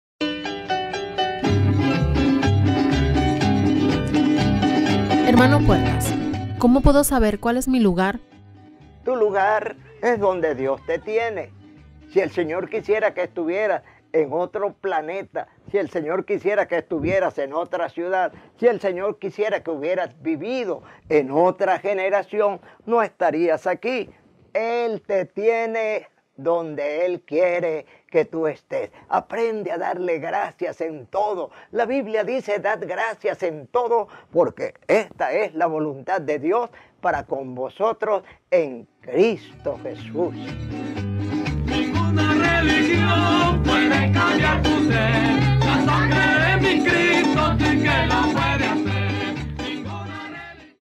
HNO PUERTAS RESPONDE #108 | ¿COMO PUEDO SABER CUAL ES MI LUGAR? | MICRO HNO PUERTAS | LUZ TV
pastor Jaime Puertas:
¿Cómo puedo saber cuál es mi lugar?